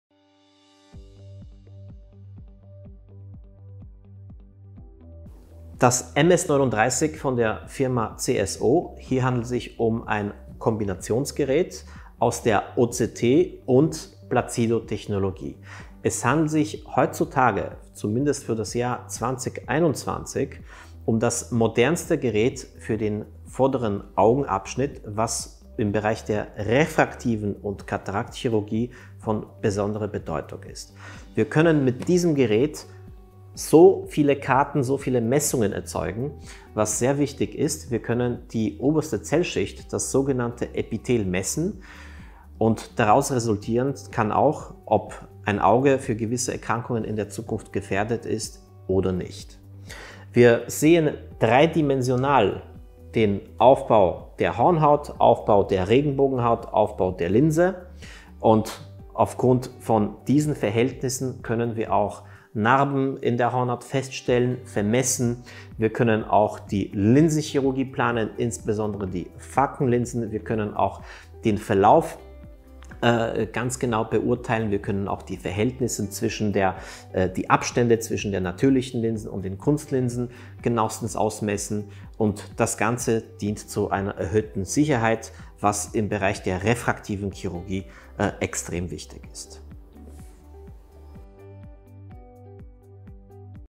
Modernste Augenlaser Diagnostik MS39 CSO
Modernste Augenlaser Diagnostik, Hornhaut Epithel Vorbereitung für augenlaser Chirurgie

Oder nehmen Sie direkt Kontakt zu unserem Eyelaser-Team in Wien auf:

EYELASER WIEN
Opernring 1, Stiege R,
5. Stock
1010 Wien